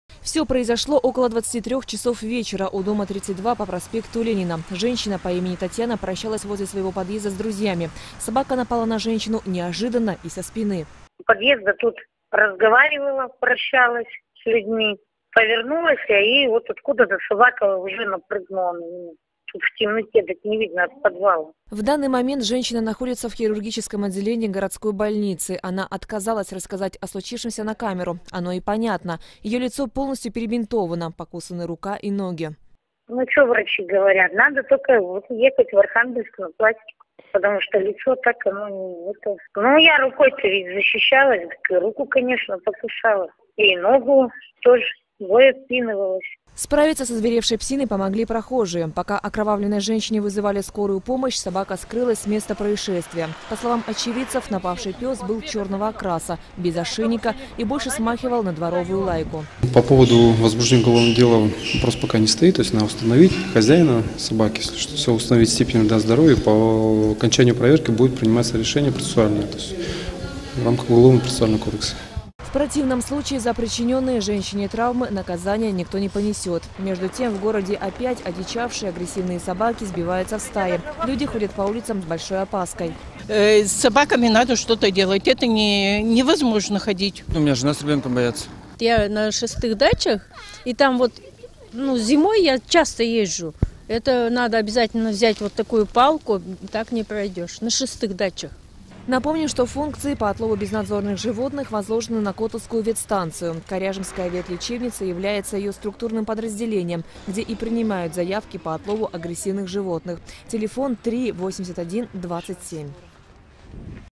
На женщину прямо у подъезда напала собака
30 ноября поздним вечером на жительницу города, 37-ми лет, напала одичавшая собака. Женщина получила серьезные травмы лица. По мнению врачей, в данном случае без пластической хирургии не обойтись.